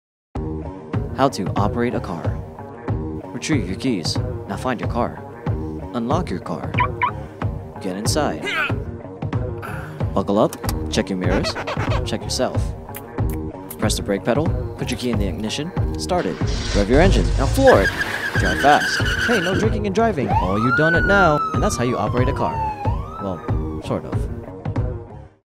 HOMEWORK-How to Operate a Car
The task was to use close-up shots ONLY. I think I passed.